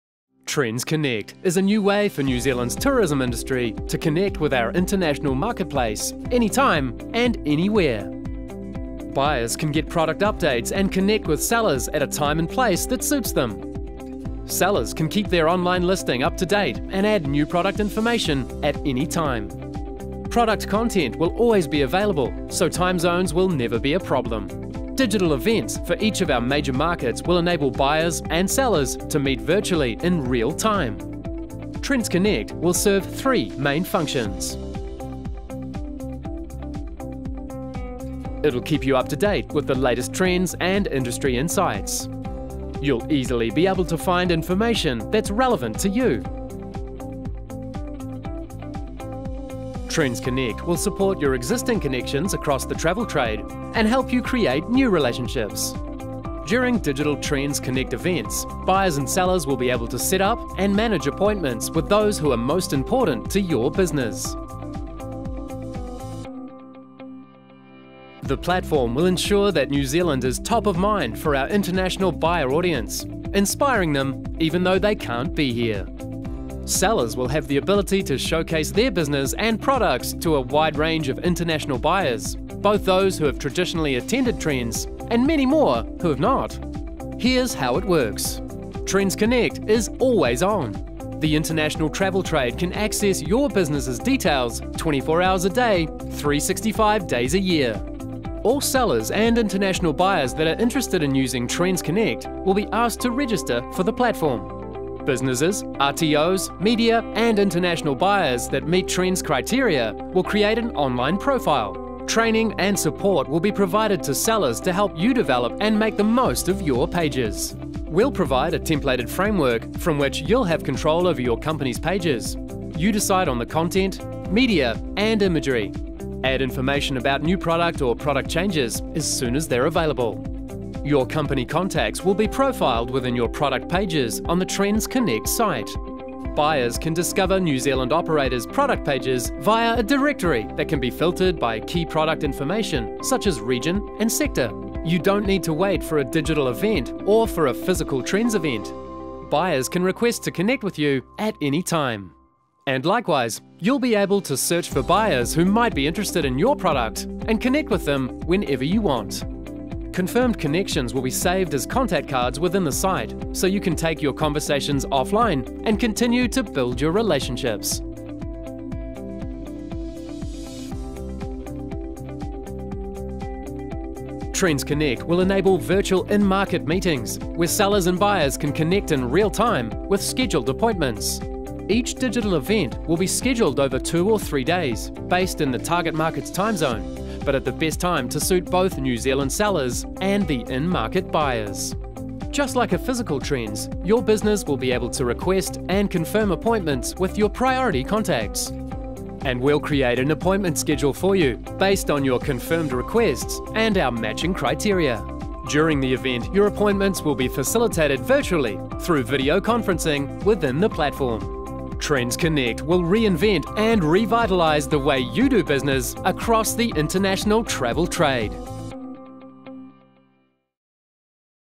TRENZ Connect
A new way for New Zealand’s tourism industry to connect with the international travel trade, anytime and anywhere, was announced at TRENZ Hui 2021.

TRENZ Connect is a digital marketplace where New Zealand’s leading tourism operators will be able to provide product updates and have virtual meetings with travel and tourism buyers from around the globe.

The new platform will allow the international travel trade to access the details of New Zealand tourism businesses 24 hours a day, 365 days a year, no matter what time zone they are in.